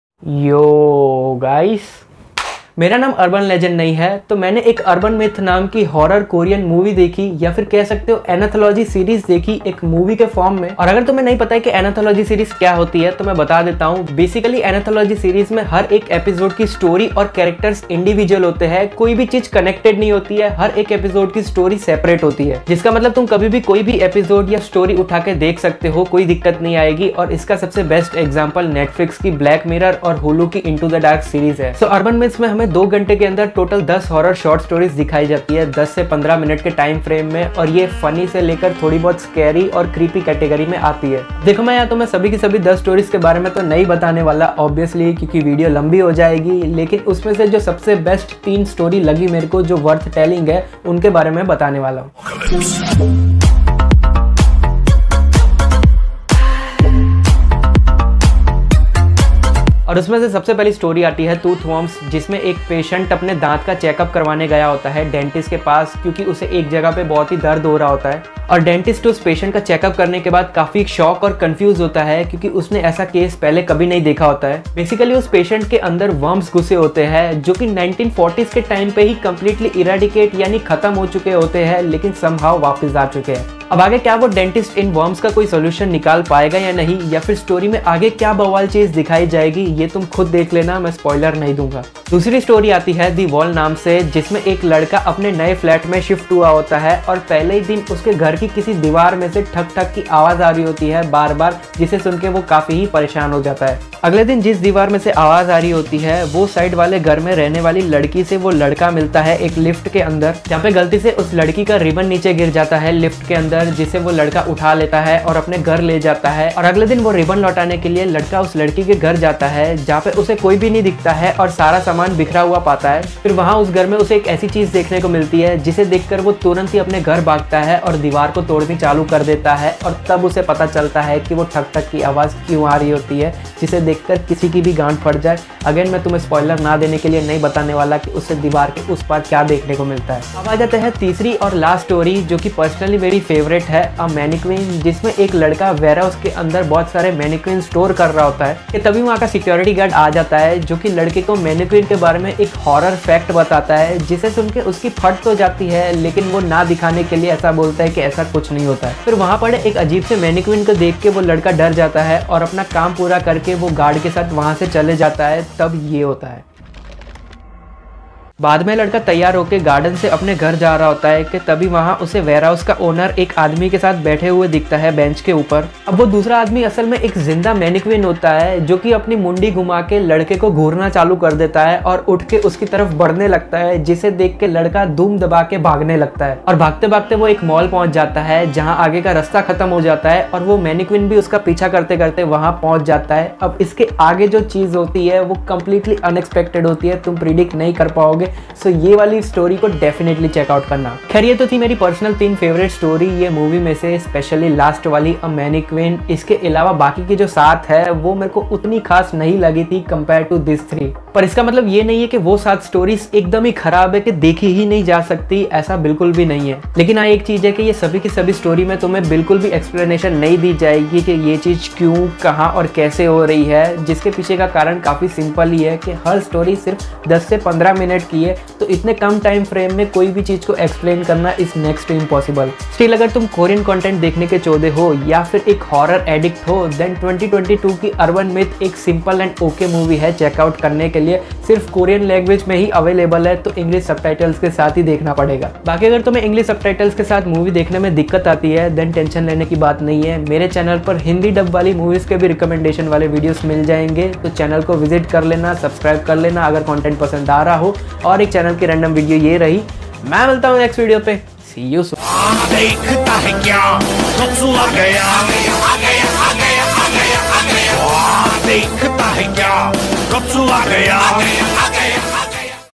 KOREAN HORROR MOVIE || Urban Myths Review in Hindi || 10 Horror Short Stories
Movie Name - Urban Myths 2022
PLEASE CHECKOUT MY OTHER WORK Need support !!